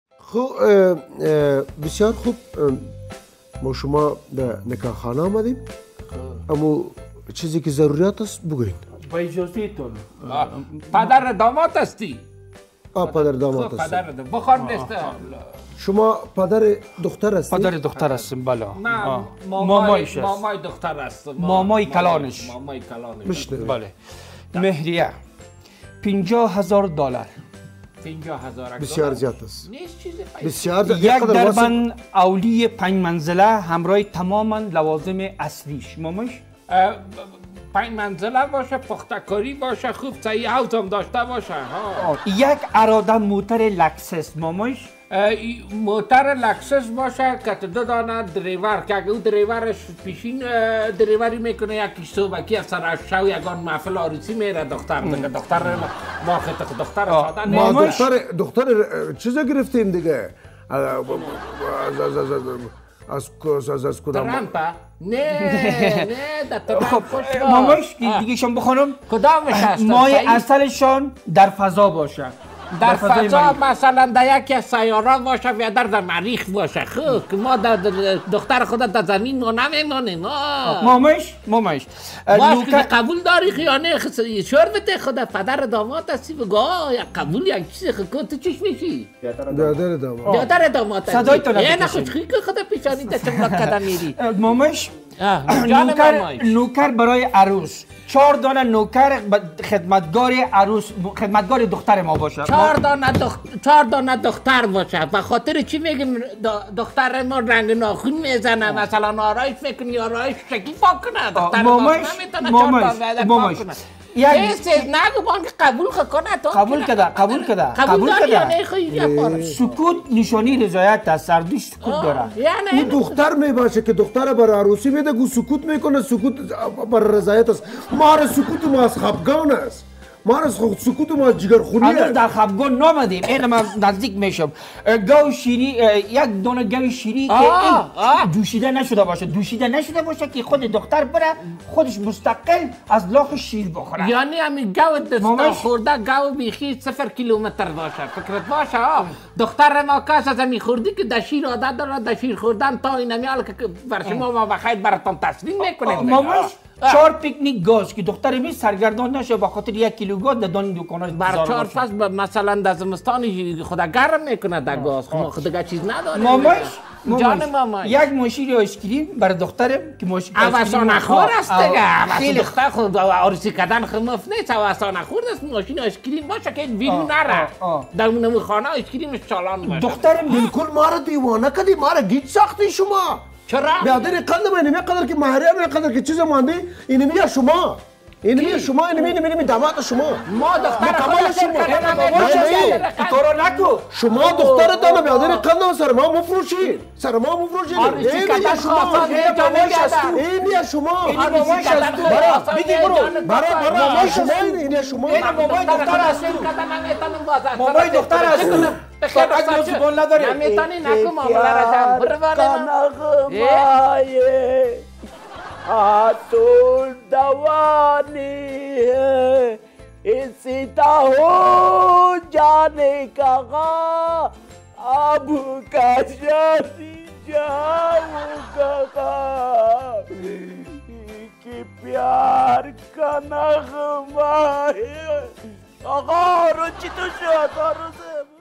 شبکه خنده - داماد که در شب عروسی مجرد شد
CLICK HERE to Experience the Joy of Cooking in Nature, in the traditional way that Afghan people do!👇

طویانه هایی که از سوی فامیل دختر تعیین گردید کمر داماد را در شب عروسی اش شکست و او را دوباره مجرد کرد.

میگویید چطور؟
ویدیو را تا آخر بیبینید.